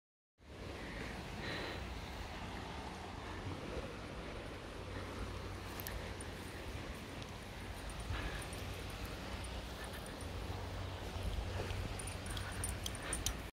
Ermis an der Salzachbrücke, 31. Juli 2020
Kokoni Ermis von Kalymnos, unser Straßenhund aus Griechenland, jetzt im Berchtesgadener Land